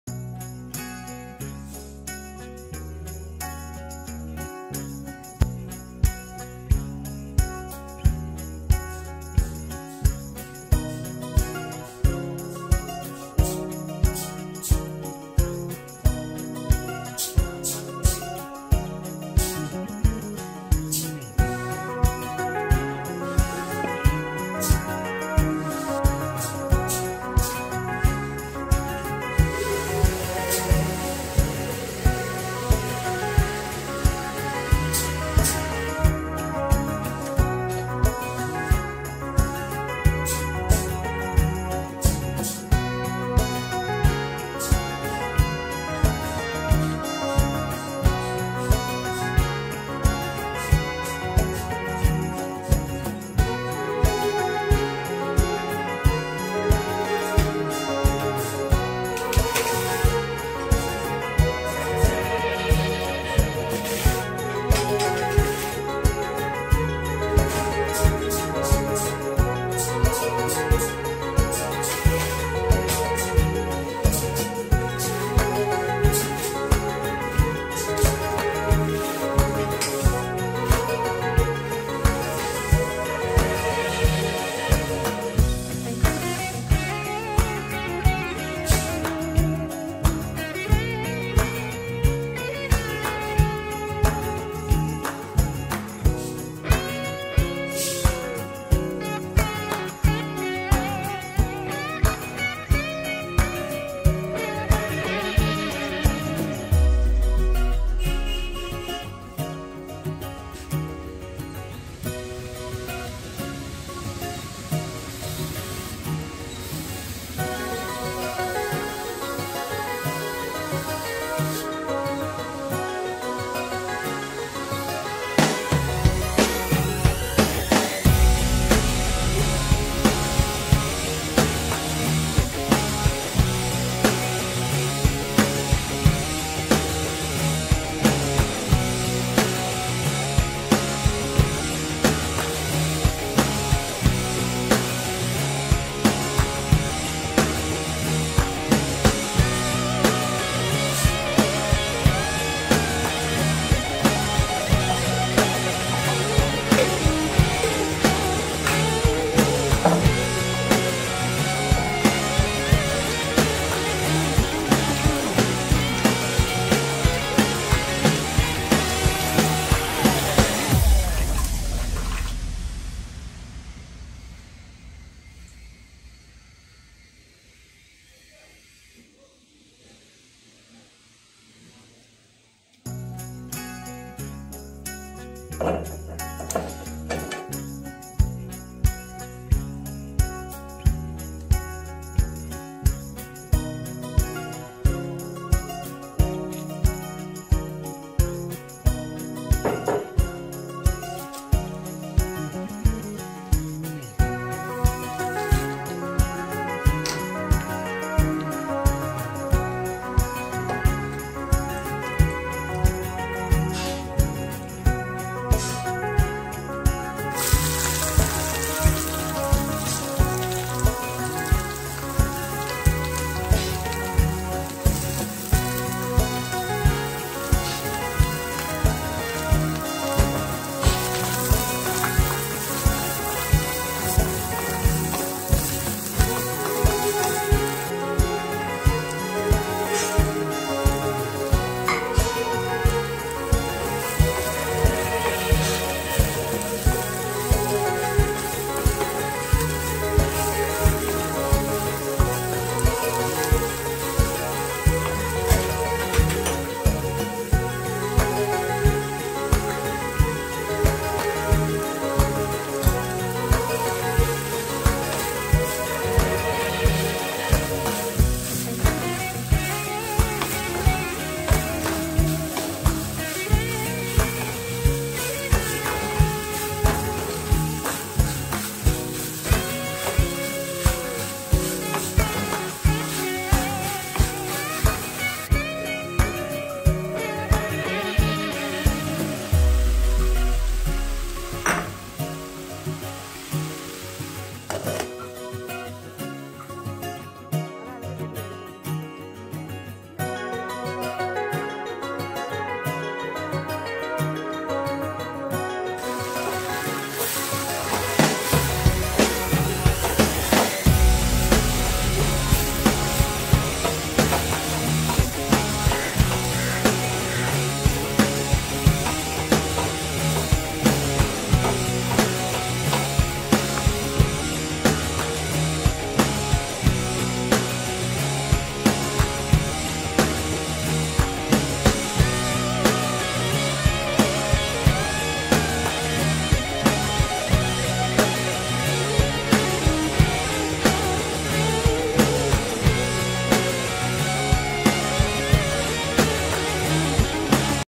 Traditional Kerala Drumstick Flower
Drumstick flower 2 cup

coconut  one cup
Greenhill  1
curry leaf
oil